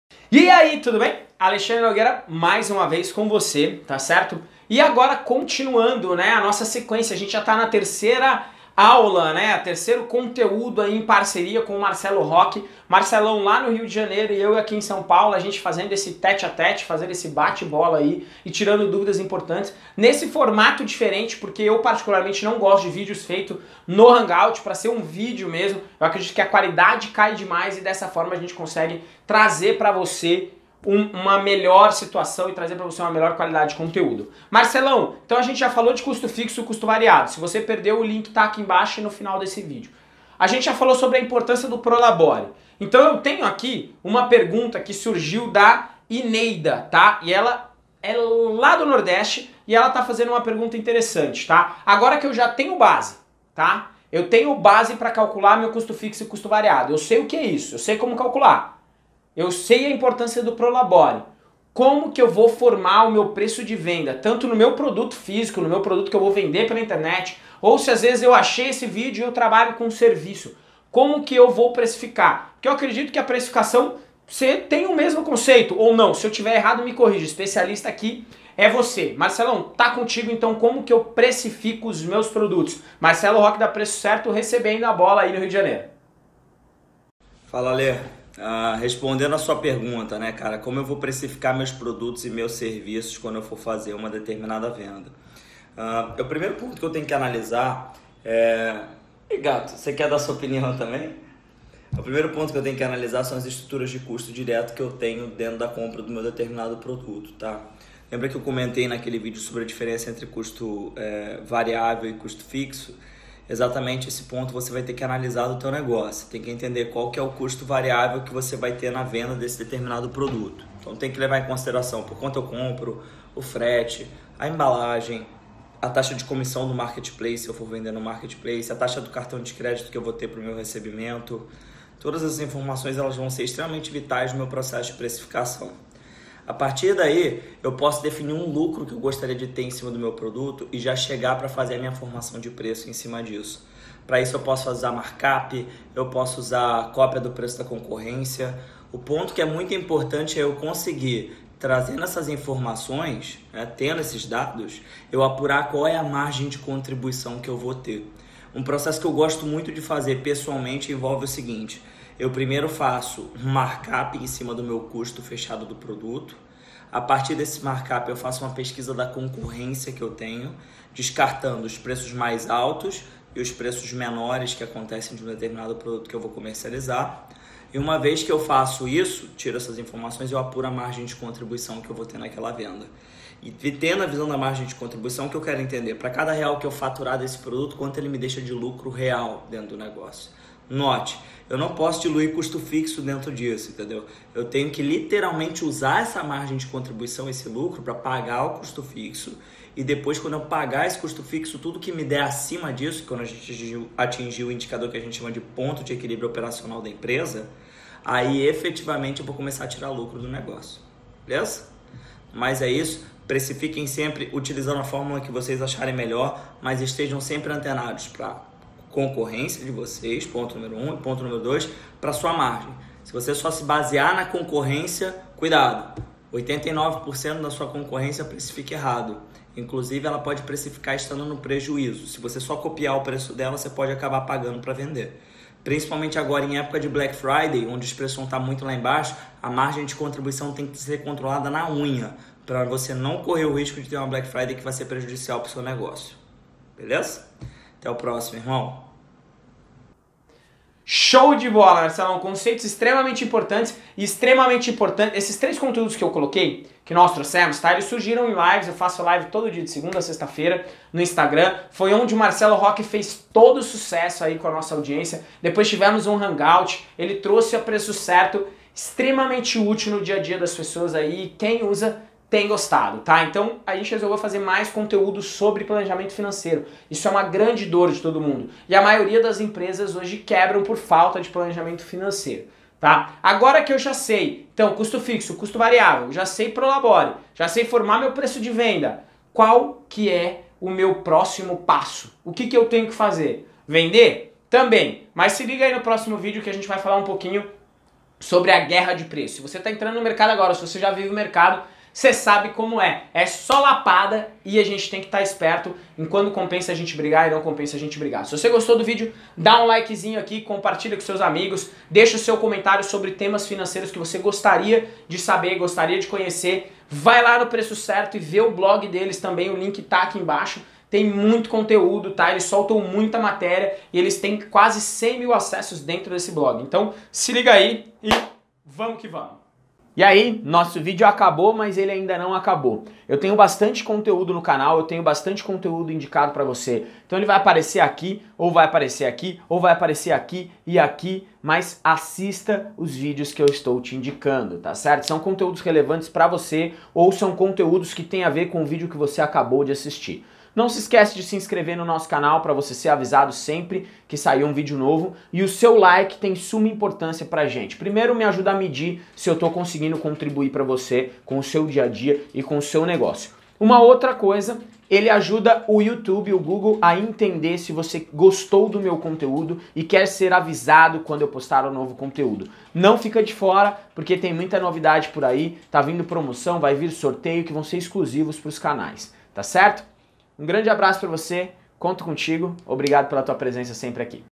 PORQUE CALCULAR PREÇO DE VENDA CORRETAMENTE MARKETPLACES E-COMMERCE PART. MARCELO ROQUE PREÇO CERTO
ACESSE ABAIXO:
TREINAMENTO GRATUITO SOBRE MERCADO LIVRE E LOJA VIRTUAL (+80 AULAS):

Conteúdo feito em parceria com a Empresa Preço Certo, especialista financeiro e em precificação de produtos para Negócios.

O Foco da Preço Certo é Facilitar a Formação de preço correta dos vendedores, evitando erros que levam a ruína e falência dos negócios em todo o Mundo.

Esta Primeira série possuirá uma sequência de 4 vídeos abordando:

- Custo Fixo e Custo Variável;
- Como Definir Pró-labore;
- Criação do Preço de Vendas;
- Entrar ou não na Guerra de Preços;

Fique ligado.

Att
Alexandre Nogueira
Fundador Faça o Seu Destino